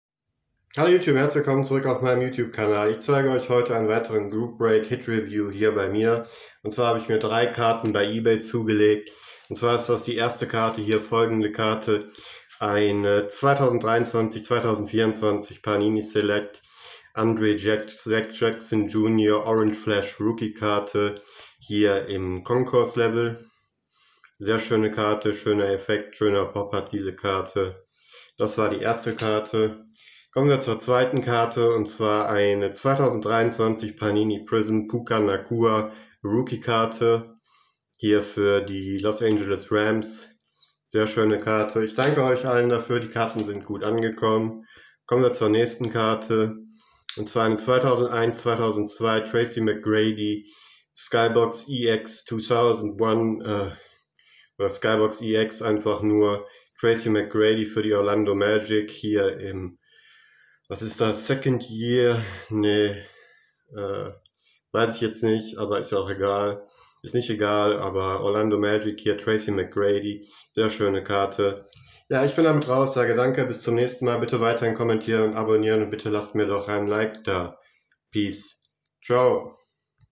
Mid Of Summer Breaks #6, Nice Rookie Card (Deutsch/German) 🚀💥🔥🏀
Hallo zusammen,

in diesem Video zeige ich euch meinen Mid Of Summer Breaks #6 Nice Rookie Card.